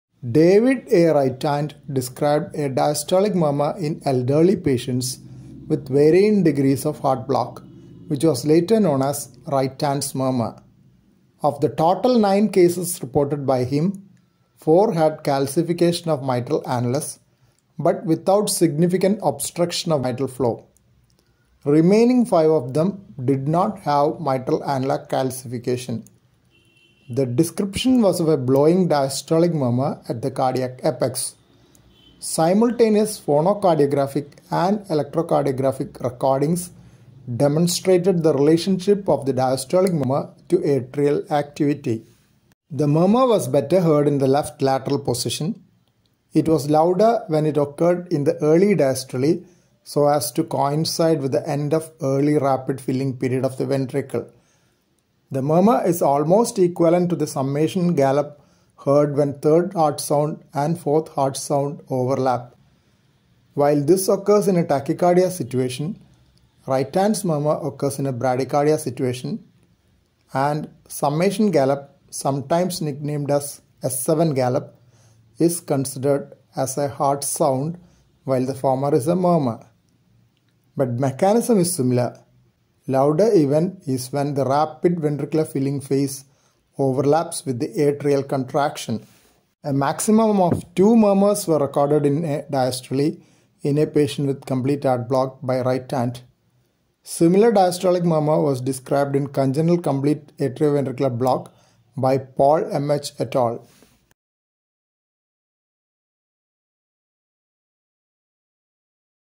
Rytand's murmur in heart block
David A Rytand described a diastolic murmur in elderly patients with varying degrees of heart block, which was later known as Rytand's murmur. Of the total 9 cases reported by him, 4 had calcification of mitral annulus, but without significant obstruction of mitral flow. Remaining 5 of them did not have mitral annular calcification. The description was of a blowing diastolic murmur at the cardiac apex. Simultaneous phonocardiographic and electrocardiographic recordings demonstrated the relationship of the diastolic murmur to atrial activity.

The murmur was better heard in the left lateral position. It was louder when it occurred in the early diastole so as to coincide with the end of early rapid filling period of the ventricle. The murmur is almost equivalent to the summation gallop heard when third heart sound and fourth heart sound overlap. While this occurs in a tachycardia situation, Rytand's murmur occurs in a bradycardia situation and summation gallop (sometimes nick named as S7 gallop!) is considered as a heart sound while the former is a murmur. But mechanism is similar - louder event is when the rapid ventricular filling phase overlaps with the atrial contraction.

A maximum of two murmurs were recorded in a diastole, in a patient with complete heart block by Rytand. Similar diastolic murmur was described in congenital complete atrioventricular block by Paul MH et al.